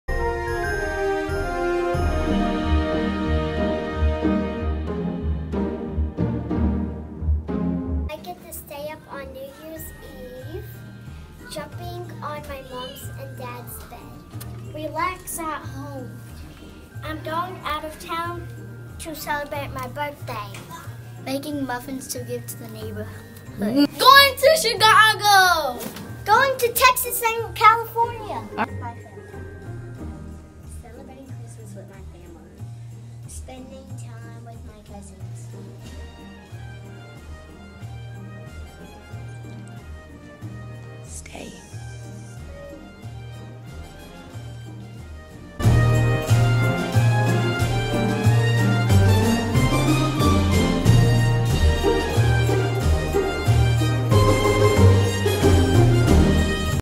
Winter Break Baker
We asked students across Pulaski County Special School District: What are you looking forward to most about winter break? Here are the responses from Baker Elementary.